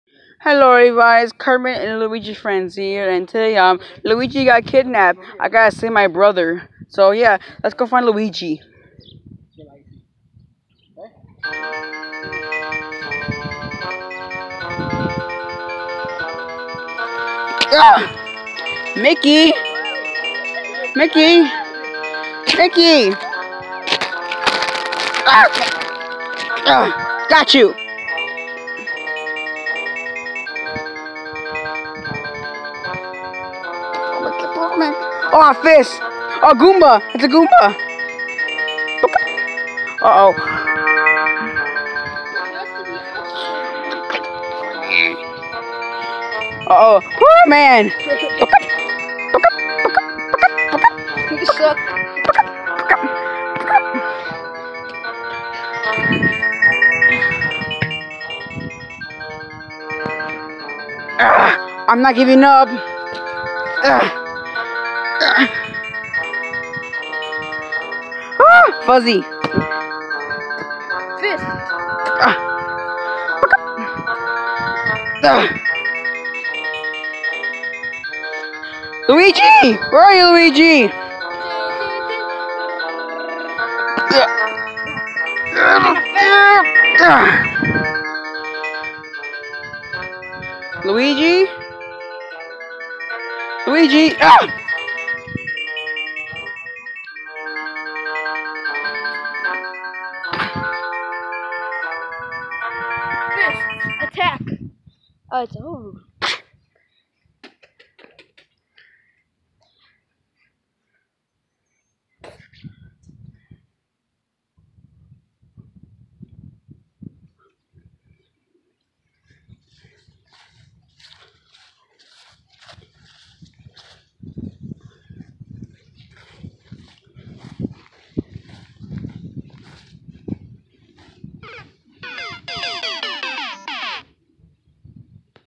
mario fails 2
mario fails again but this time luigi got kidnapped